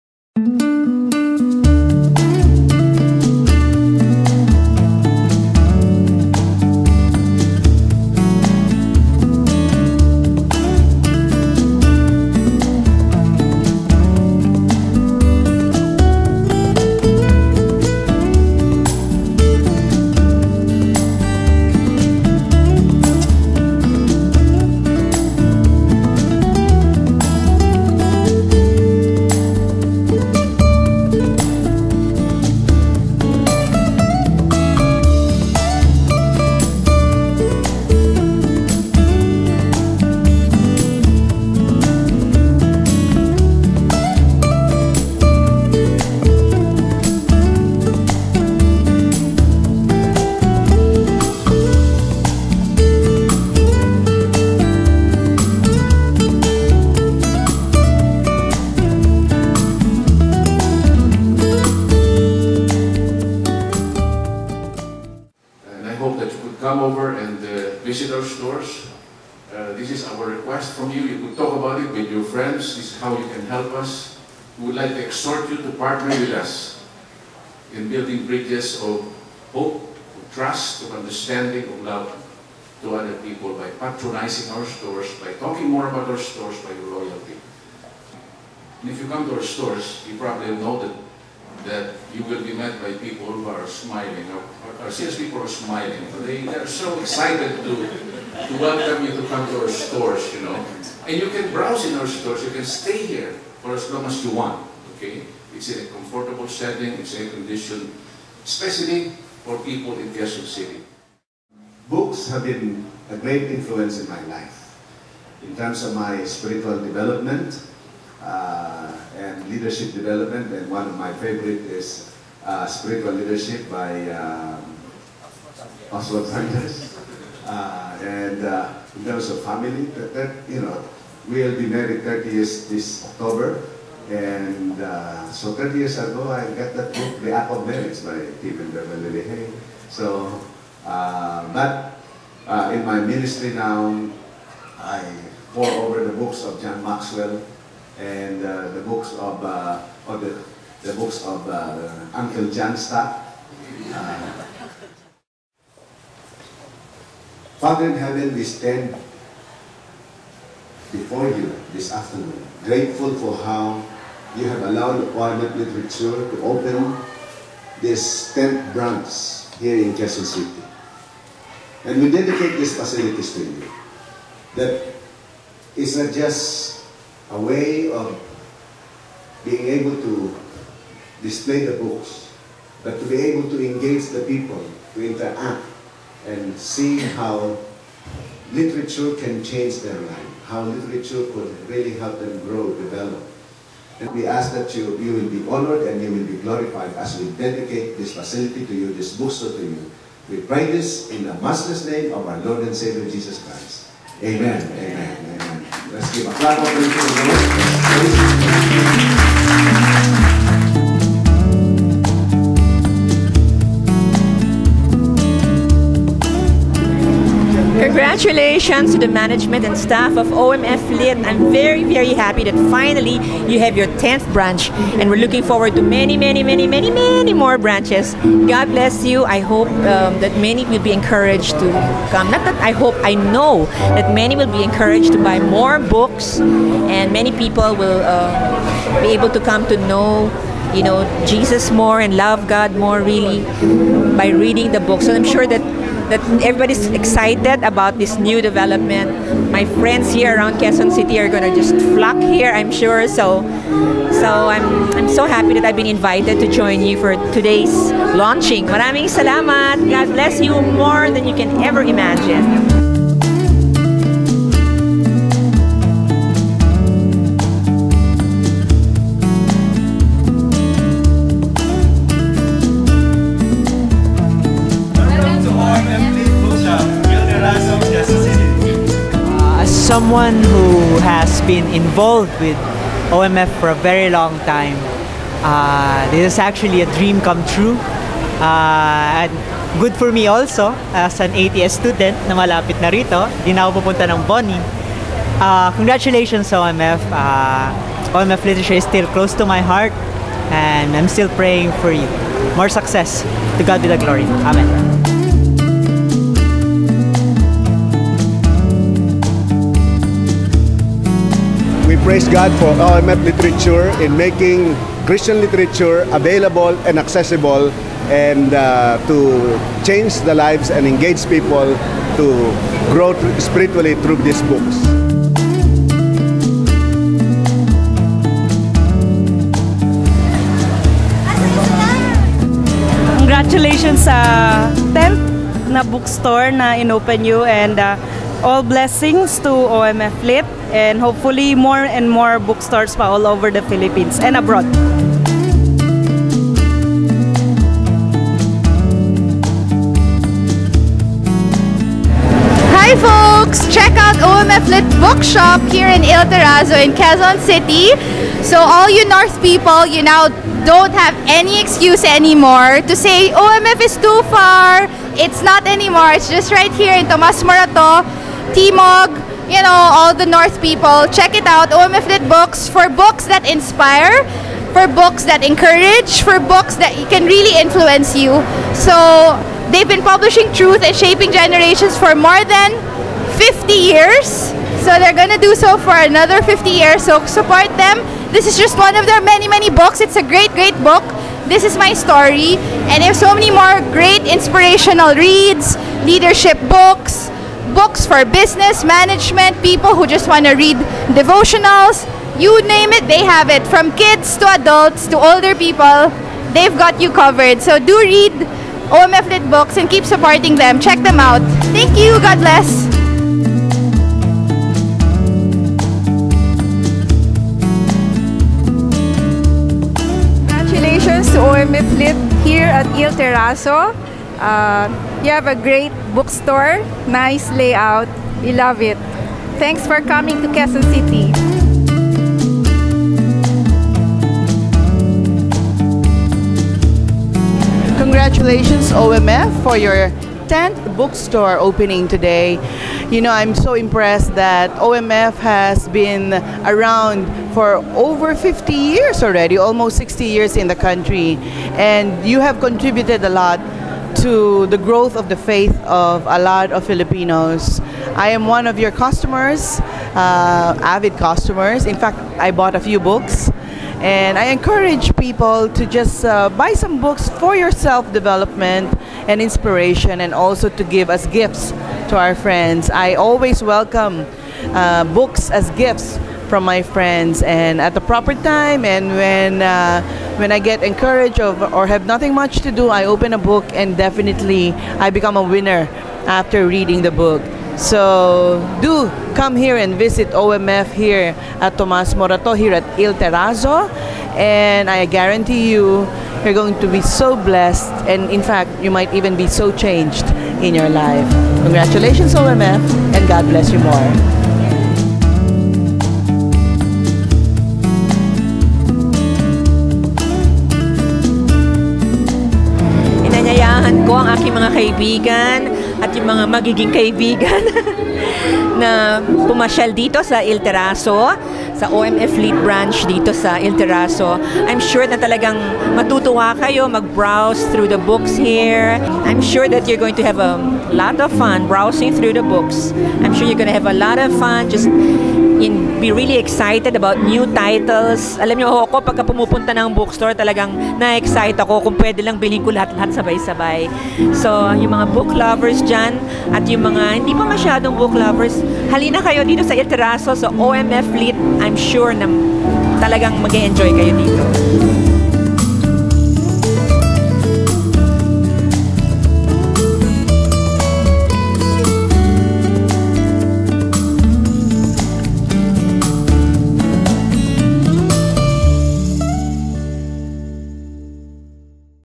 OMF Lit Bookshop Il Terrazzo, QC Grand Opening
The OMF Lit Bookshop celebrated the grand opening of its branch in Il Terrazzo, Tomas Morato cor. Sct. Madrinan, QC on July 10, with special guests Coney Reyes and Kata Inocencio.